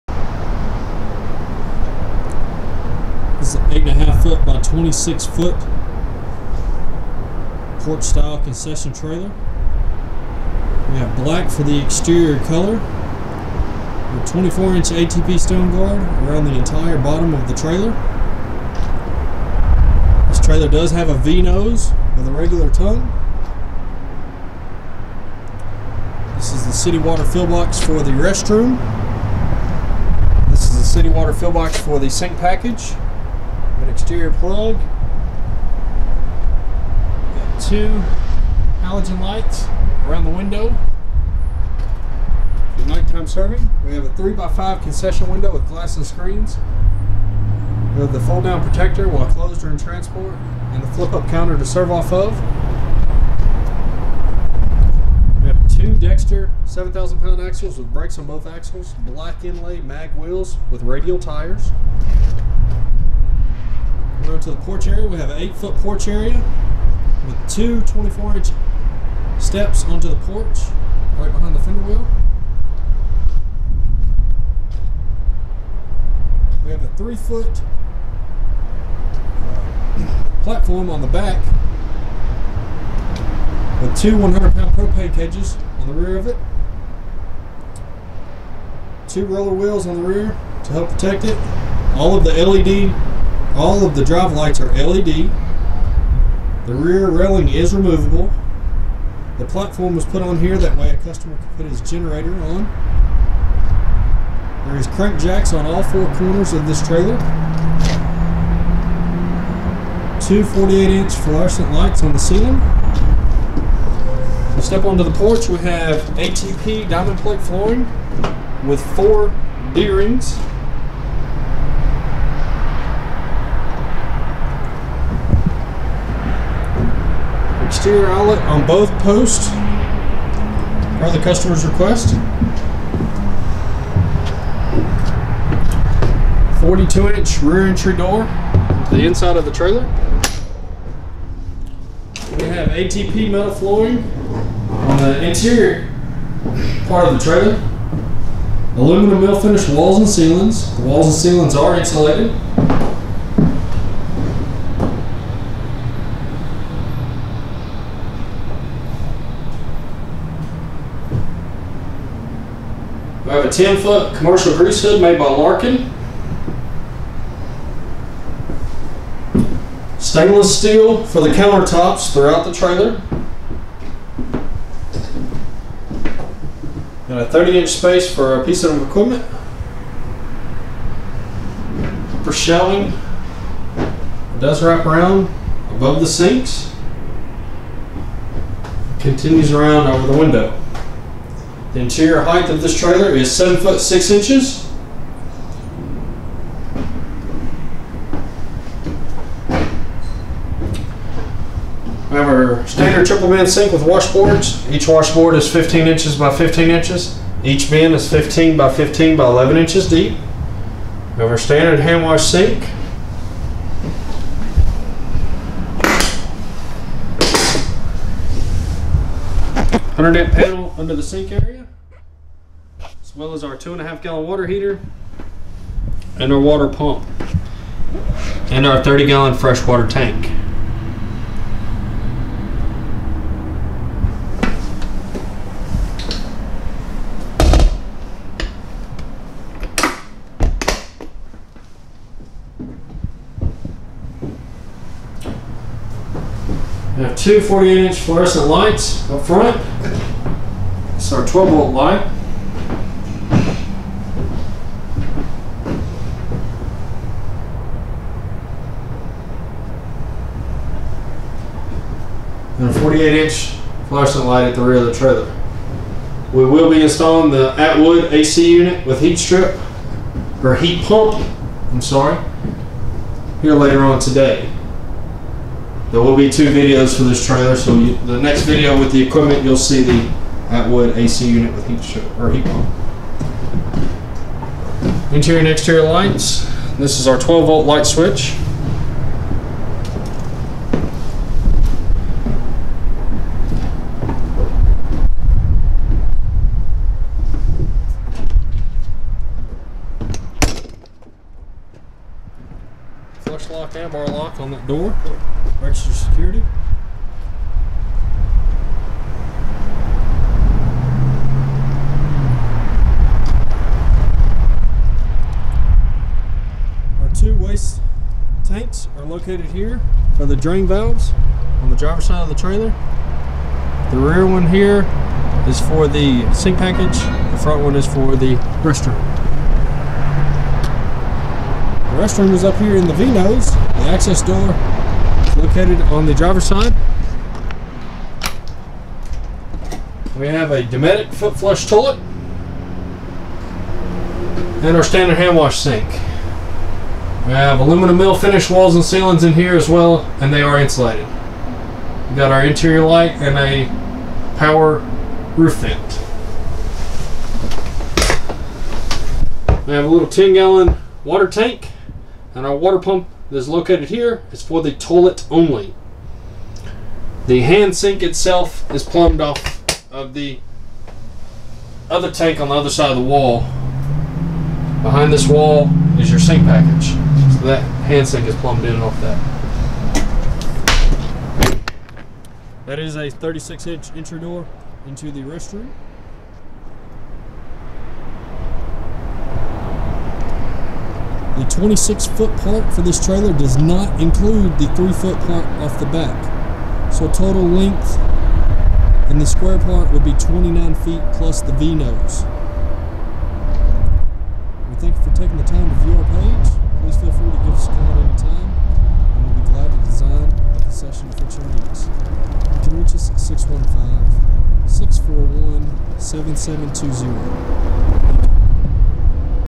Concession Trailer 8.5' x 28' Black Catering Event Food Trailer
Features and Specifications:

-8.5' X 28' Porch Style Concession with 36'' Extended Platform for the Main Frame but Not Covered
-7.5' Interior Height
-Insulated Walls and Ceiling
-Aluminum Mill Finish Walls and Ceiling Throughout
-Sink Package with 30 Gal. Fresh, 50 Gal. Waste, 2.5 Gal. Water Heater
-City Water Fill Box
-Steel Spare Wheel and Radial Tire
-ATP Metal Flooring Throughout
-100 AMP Electrical Package
-Crank Jacks on All 4 Corners
-Black for Exterior Color
-(2) 7,000 lb Axles with Brakes
-Restroom with Toilet, Hand Sink, Light, Roof Vent and to Have Its Own Fresh and Waste Water Tank for the Toilet
-5' Concession Window with Glass and Screens with Flip Up Counter Outside
-10' Commercial Grease Hood
-Cabinet and Shelving Package
-(2) 100 lb Propane Cages on Porch
-42'' Full Height Entry Door
-Stainless Steel Counter Tops Throughout (Even the Outside Flip Up Shelf)
-Black Inlay Mag Wheels with Radial Tires
-24'' ATP Stone Guard Around the Entire Bottom of the Trailer Due to Height
-Roller Wheels on Rear